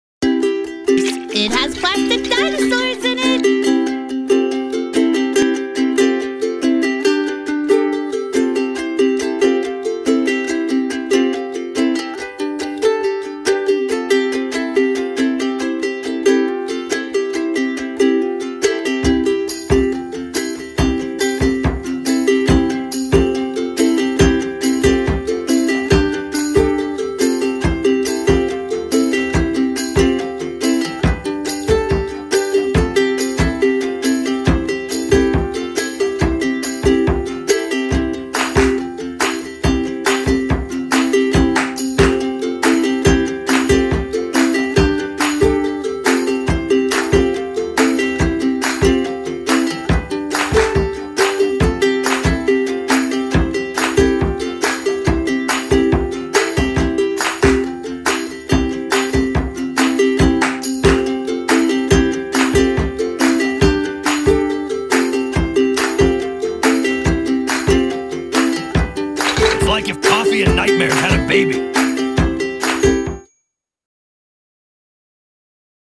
EASY TREAT: DIY Mabel Juice Energy Drink
Mabel Juice! It's glittery, sparkly and has plastic dinosaurs! Make this no-fuss energy drink as an after-school treat.

SKILL LEVEL: Easy as Pie

See even more geeky treats, cakes, cookies, recipes and more at Nerdachecakes.com

Gravity Falls is a copyright of Disney. I assume no rights over any characters. I do assume to right to drink copious amounts of sugar though.